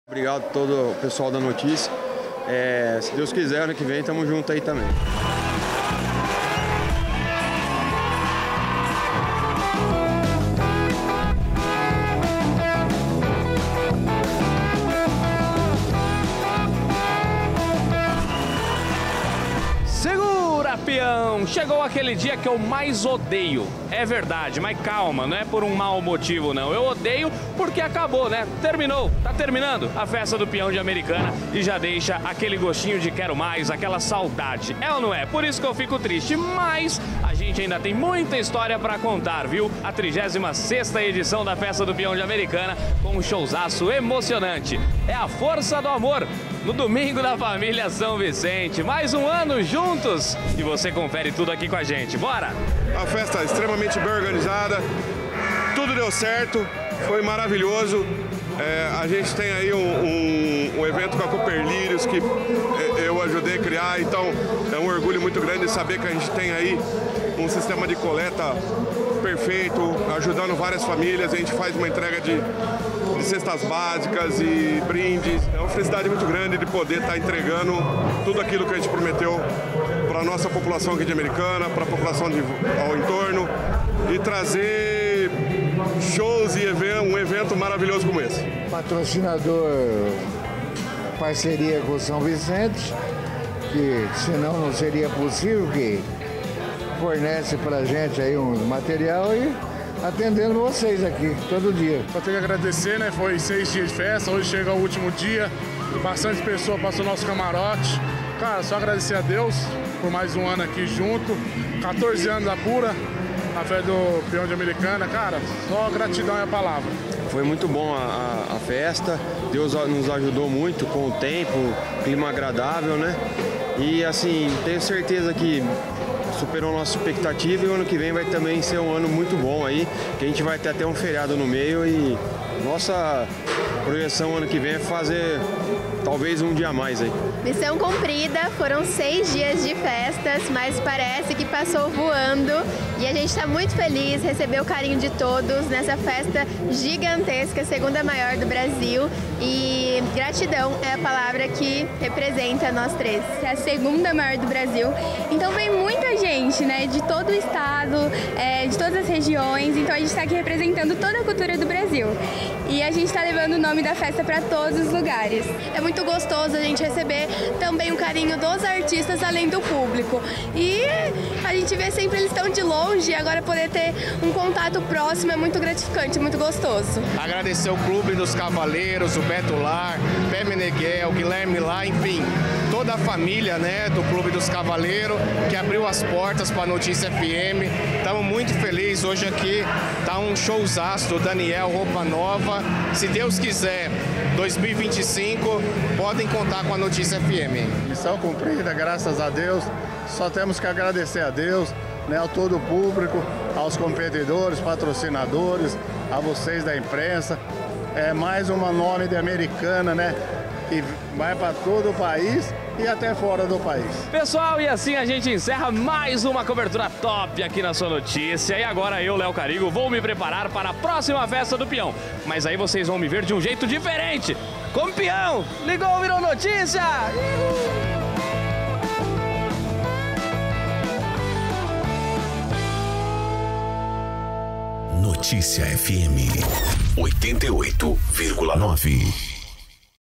🎉 A 36ª edição da @festadopeaodeamericana foi simplesmente incrível!
🎉 A 36ª edição da @festadopeaodeamericana foi simplesmente incrível! Durante 6 dias intensos, o público viveu experiências inesquecíveis ao som de shows sensacionais que fizeram todos cantar e se emocionar.

Foi um recorde de público e de shows que transformaram essa festa em um evento imperdível! Quem esteve lá sabe que essa edição vai ficar guardada no coração para sempre. Afinal, só a Festa do Peão de Americana proporciona momentos tão especiais!

E o último dia foi ainda mais marcante: Domingo da Família com o show "A Força do Amor" de Roupa Nova e Daniel, patrocinado pelo Supermercados SV.

Estamos ansiosos para reviver tudo isso! ❤ Não perca os próximos eventos e junte-se à tradição do rodeio brasileiro com a Festa do Peão de Americana! 🤠🎶

Ligou, virou Notícia!!! 💚💙